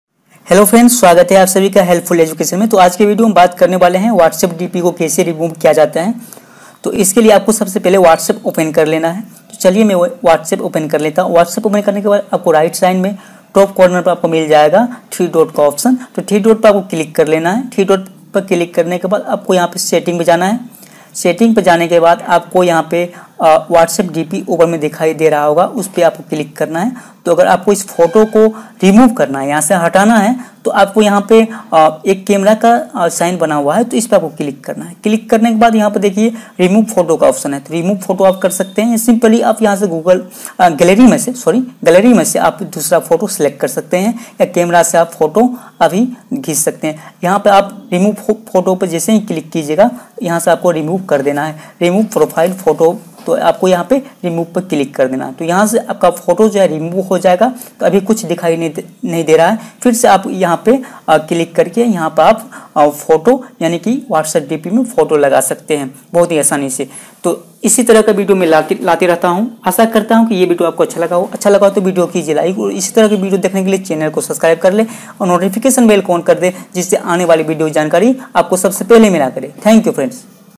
Whatsapp Profile Picture Kaise hataye | How to remove whatsapp dp | whatsapp dp ko kaise delete kare
Dosto Aaj ki is Video me, Maine Aapko Bataya hai ki app Whatsapp DP Ko Kaise Remove kare.
HELPFUL EDUCATION
Helpful education
helpful education
Helpful Education